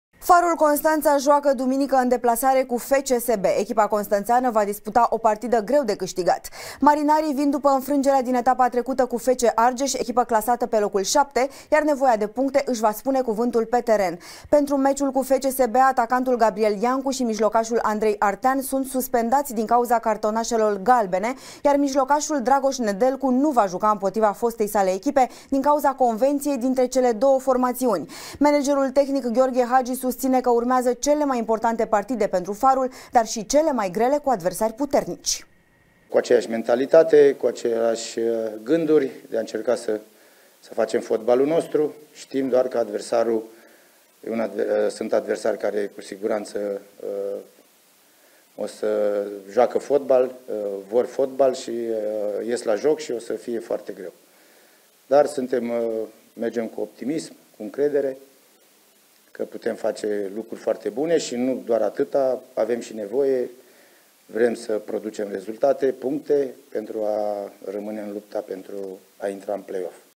Meci greu pentru Farul în deplasare
Farul Constanța joacă duminică, în deplasare cu FCSB. Echipa constănțeană va disputa o partidă greu de câștigat, în etapa a XXVIII-a a Ligii I.